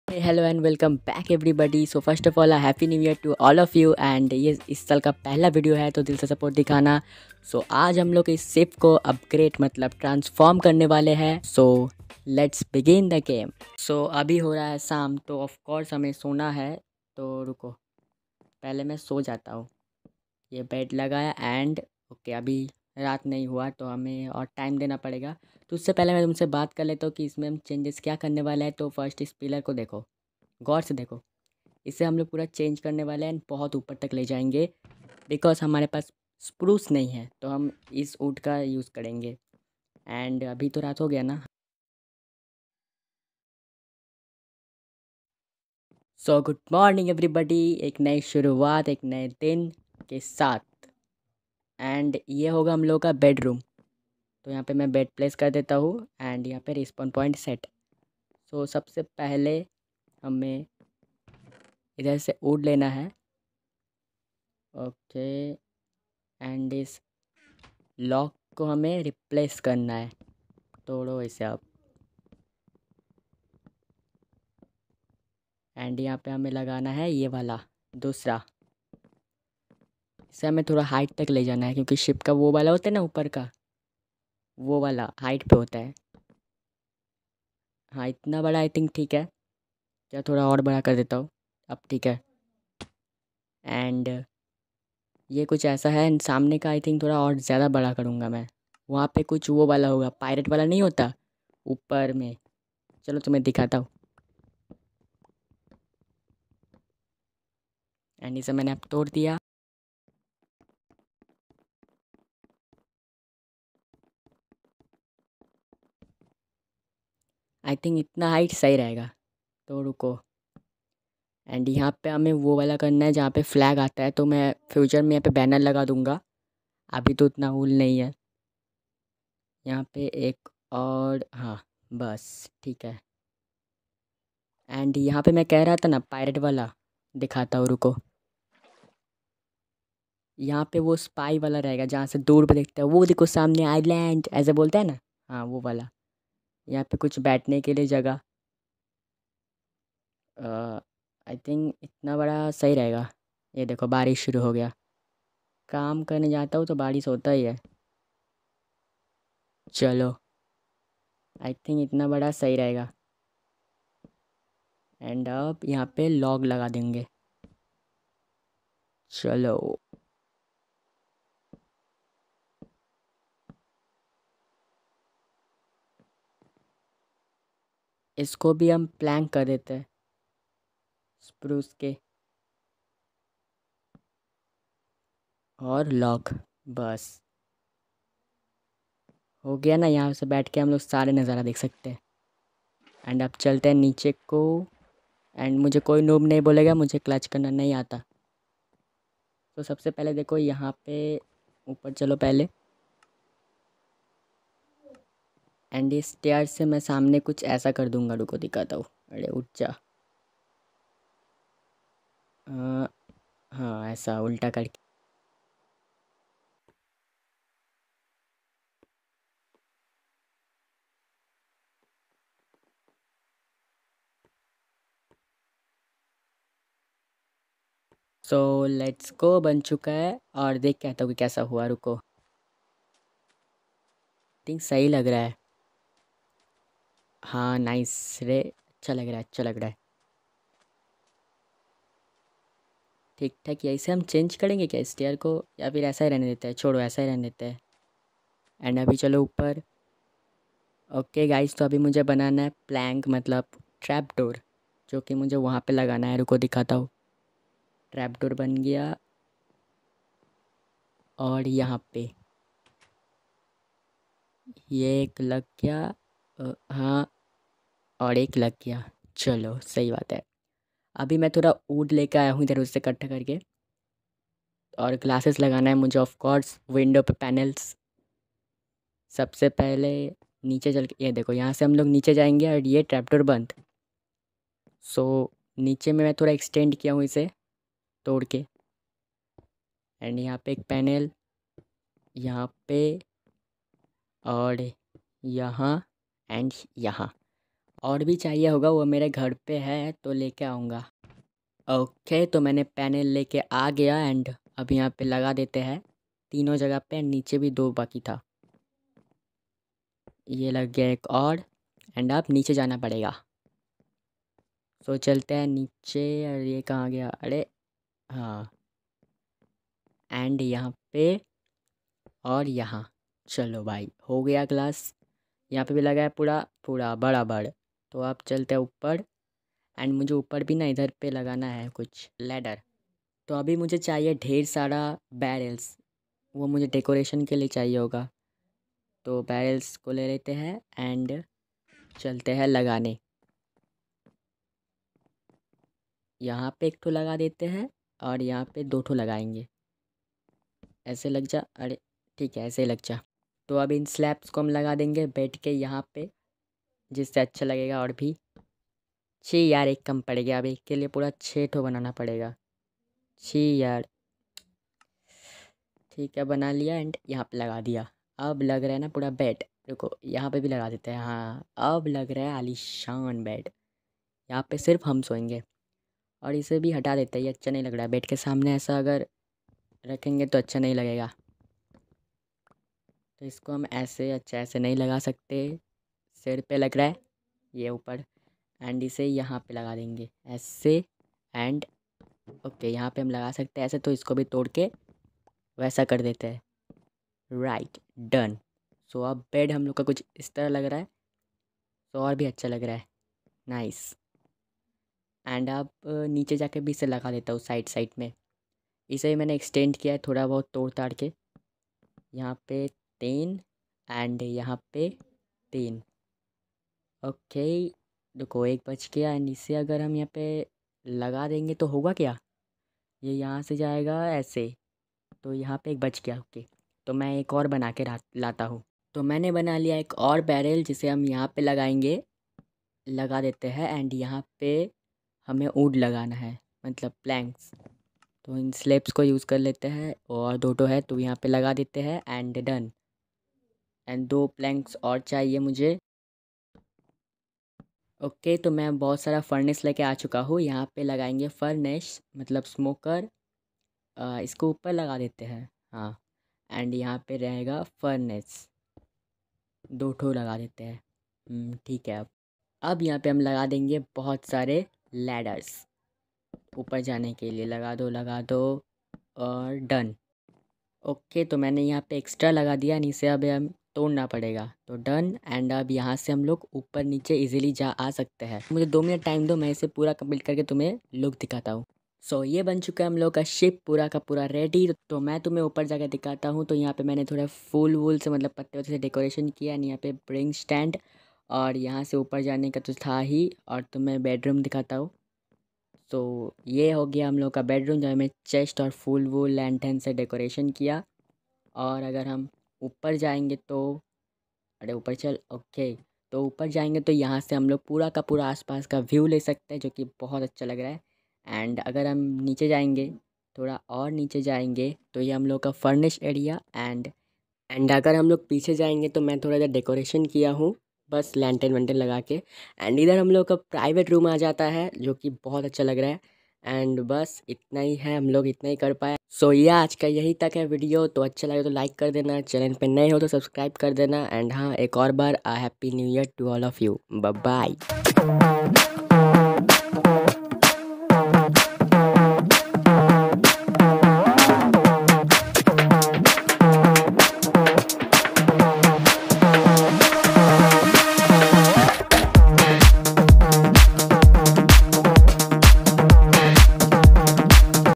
CHALO KARE TRANSFORM (SHIPWRECK) | STEBIN GAMES
Hey Hello Guys, To ajj me naya look Dene Jaa rha hu ek ship ko to jure raho end tak...

WISH YOU A VERY HAPPY NEW YEAR TO ALL❤️